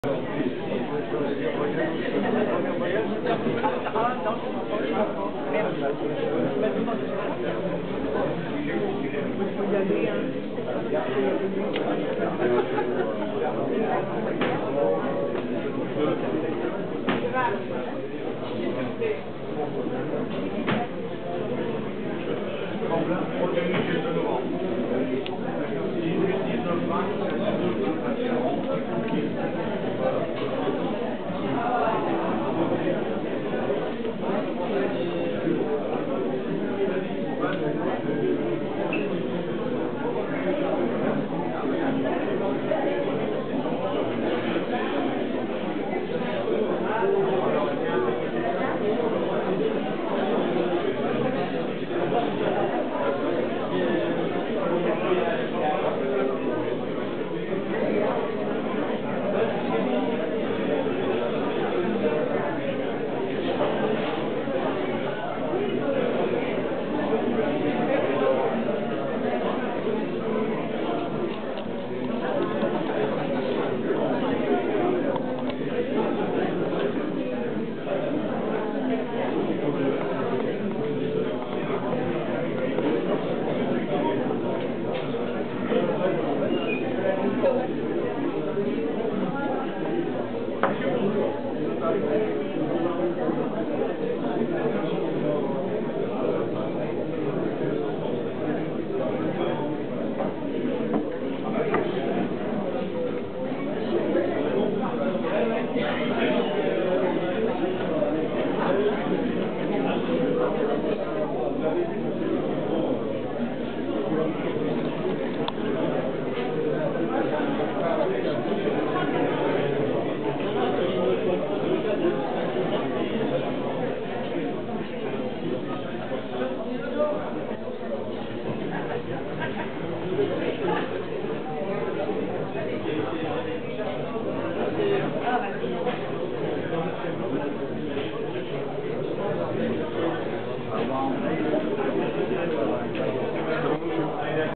Mazingarbe,La mine la vie de nos mineurs
Expo salle des fêtes de Mazingarbe le 01 Oct 2011.